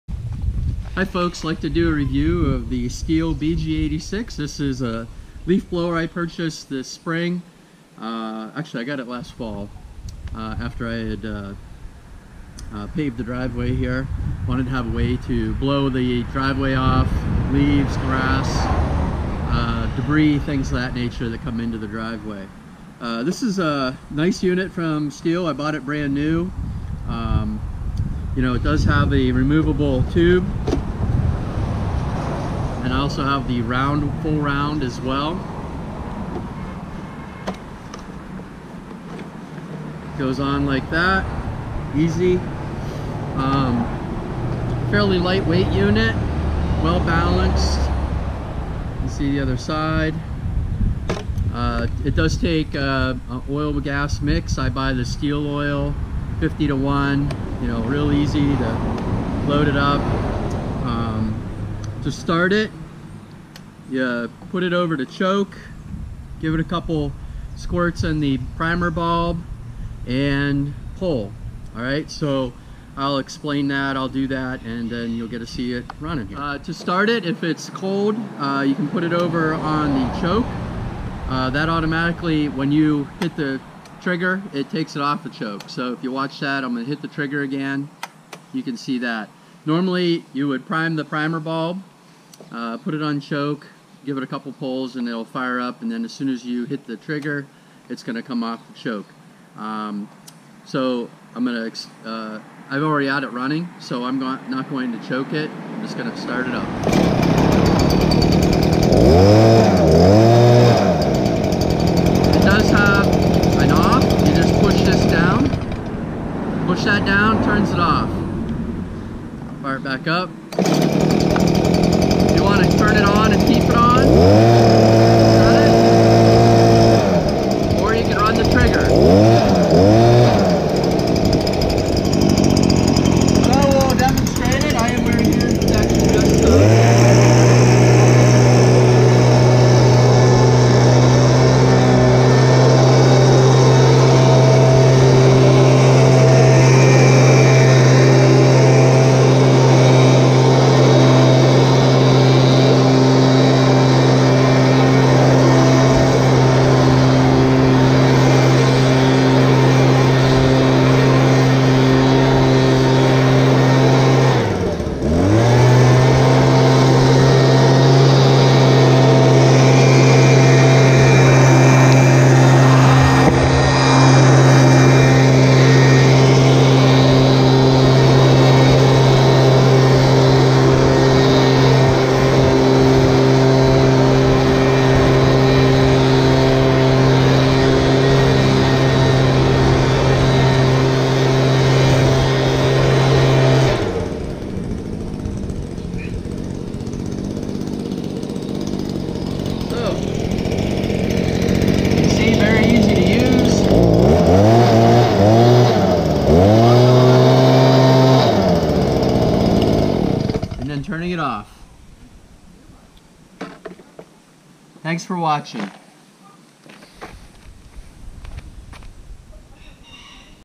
Stihl BG86 Leaf Blower
A quick review and use of the Still BG86 Leaf Blower.  I needed something to clear my carport and newly paved driveway.  Wanted something that made quick work of things but easy to handle.  This unit has low vibration, low noise, and a lots of power.  I am very pleased with it after several months of owning it.

Do you have comments or want some more videos of this unit?